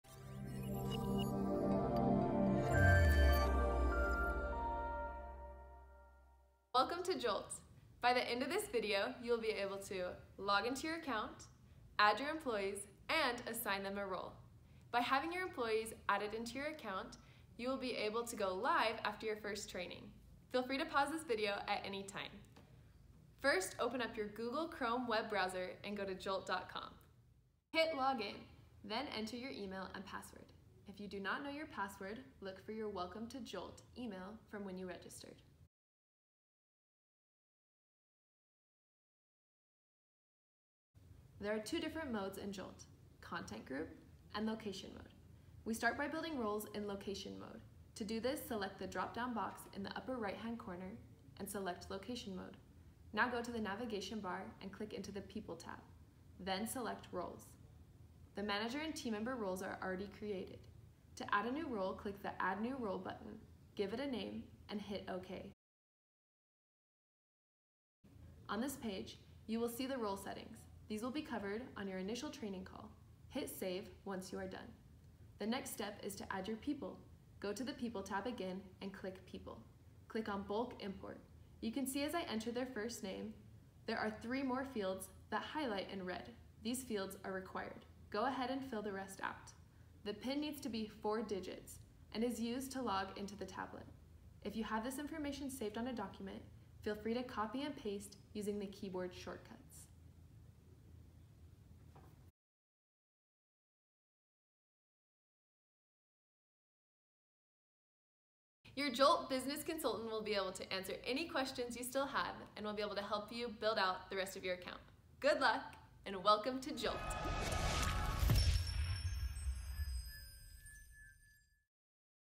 Getting Started with Jolt
How to get started with Jolt explained by Customer Success.